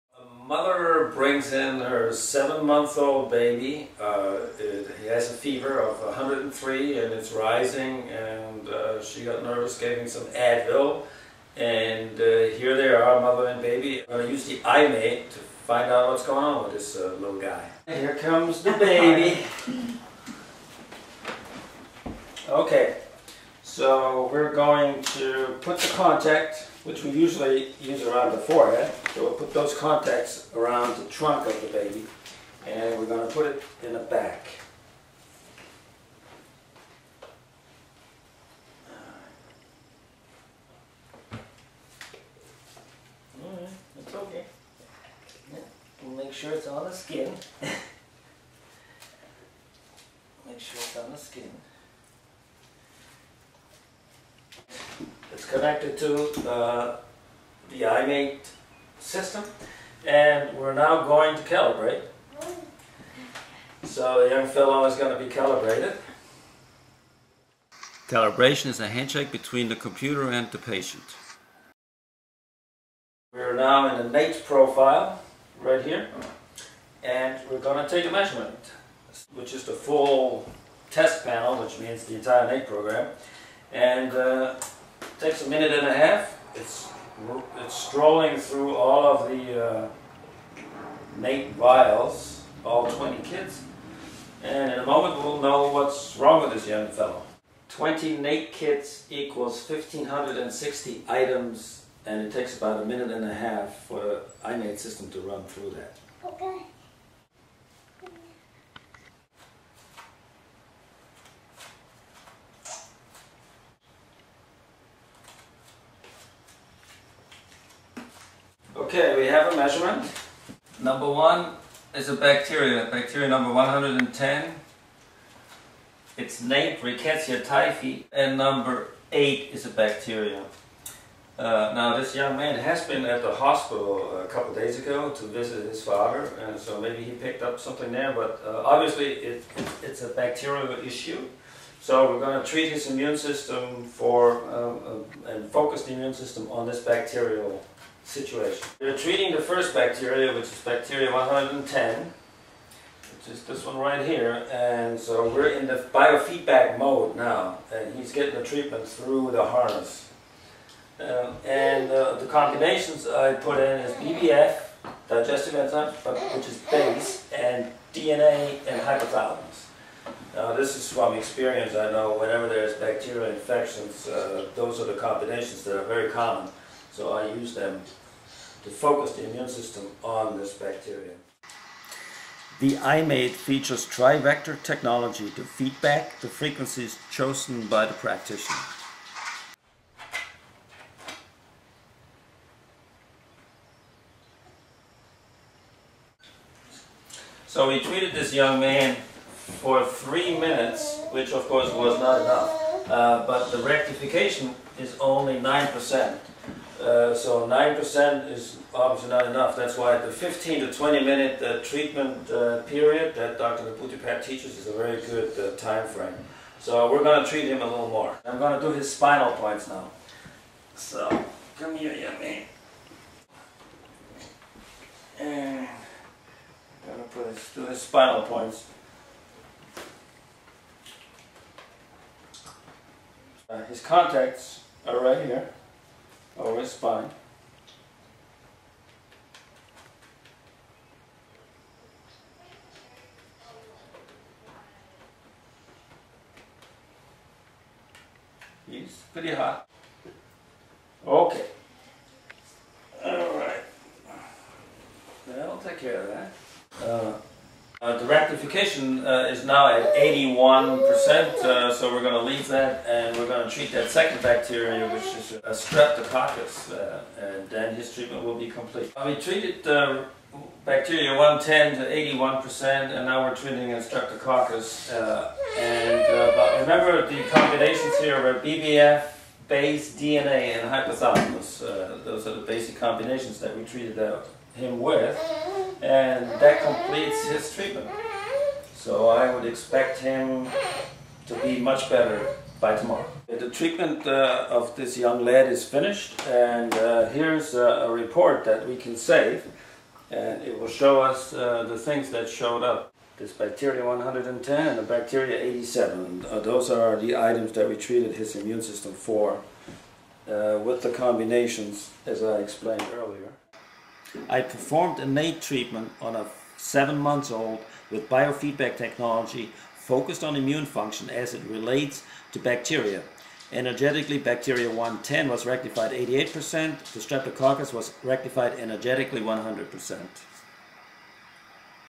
IMAET, immune modulation on a 7 month old.
7 month old baby and Quantum Biofeedback.
Immune Enhancement through Quantum Biofeedback applying NAET Profile.